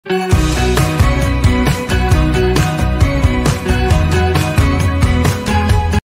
พ่อชูชก บ้านตบนม ตบหน้า ตบสั่งสอน
ป๋าอันพ่อชูชกที่ออกสื่อมากที่สุดในโลก กุ้งเขมมิกา🙏กราบขอบพระคุณ สื่อทุกแขนง สื่อทุกประเทศ 👉ไม่ว่าจะเป็น GTV รอยเตอร์ เนสเจอรัลจีโอกราฟฟิก สื่อทีวีจีน สื่อทีวีอังกฤษ สื่อทีวีเยอรมัน สื่อทีวีประเทศไทย และอีกหลาย ประเทศทั่วโลก 🌎ที่ให้เกียรติมาถ่ายทำนำเสนอ พ่อชูชกบ้านคุณกุ้งเขมมิกา กับ พลังรักพลังศรัทธา 📲 📲 ทุกความเชื่อล้วนมีที่มา 👉วันที่5เดือนพฤษภาคม2564 กุ้งเขมมิกา งดจัดงานวันเกิด 🎂นะคะ